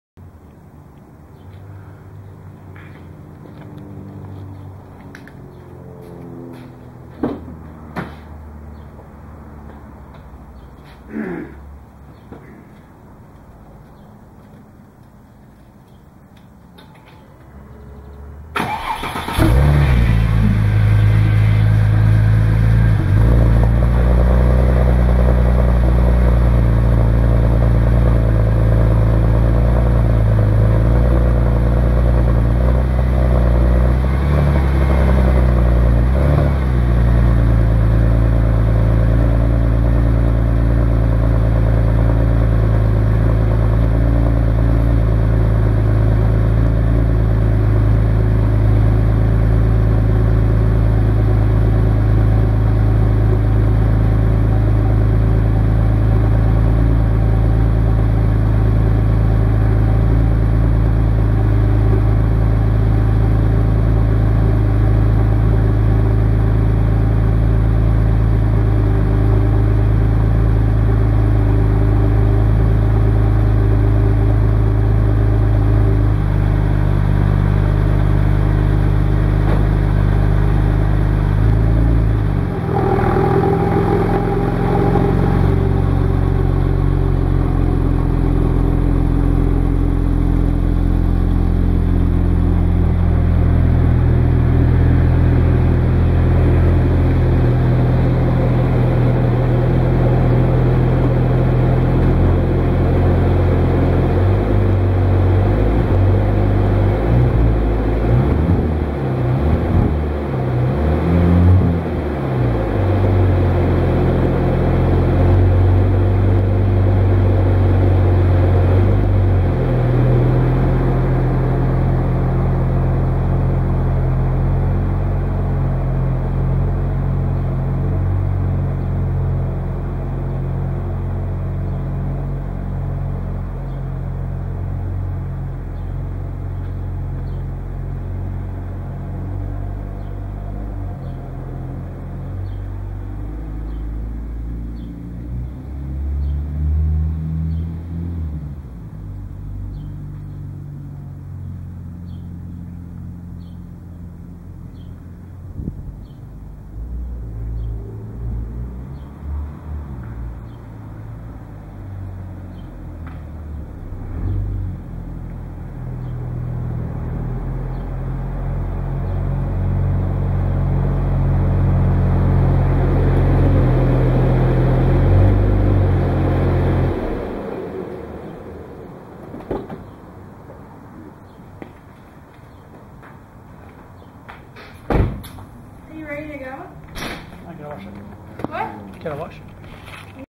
(14 of 14) Stage 2 install First Start up - Porsche 996TT TiAL Billet Turbo, Kline Exhaust
First Start up - No oil leaks! First road test results: holy cow! this thing's a beast now! ... Stage 2 Install of Porsche 996TT TiAL Billet Turbo, Kline Exhaust, & 200 cell Cats, Stainless Custom Headers, Wagner Intercooler & Hoses, 5psi FPR, Bosch Coil / Plugs.